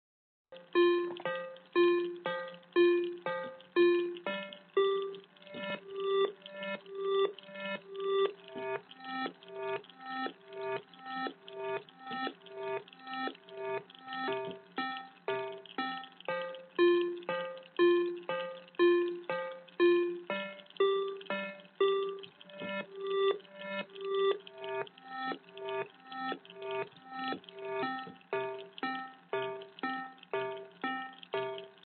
Arduino + AY8910 - Envelope testing
Some test I conducted with the envelope modification... so far only 2 envelopes work... I think illl need to change the fine and coarse values depending on the envelope, but not sure.